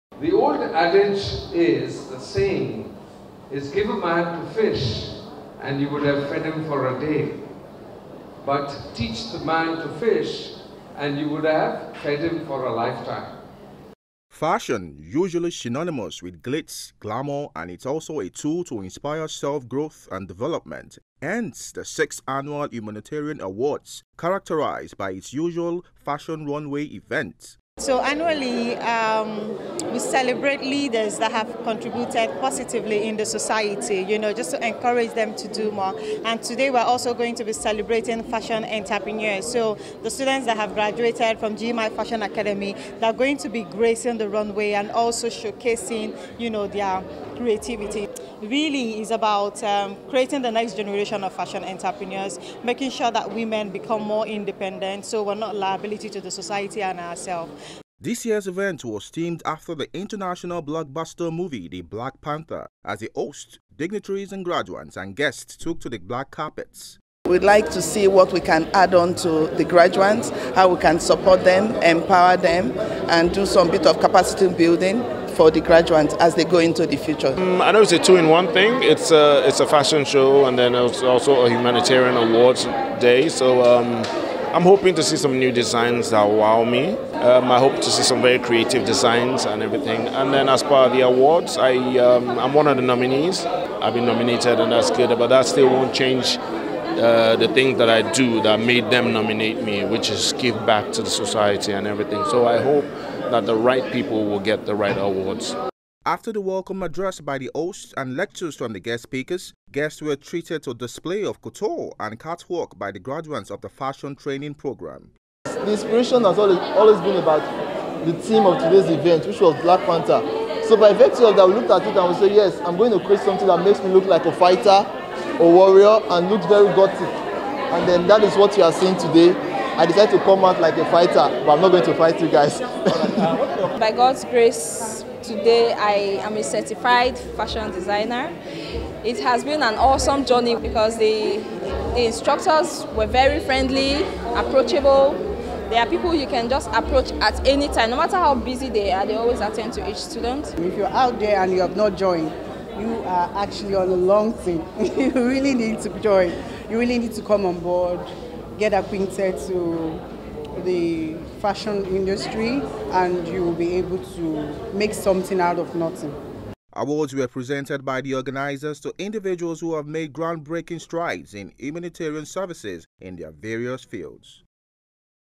GYMT Fashion and Humanitarian Awards 2019
GYMT host a graduation ceremony fashion runway show and also celebrates And awards various Humanitarian in their 6th edition 2019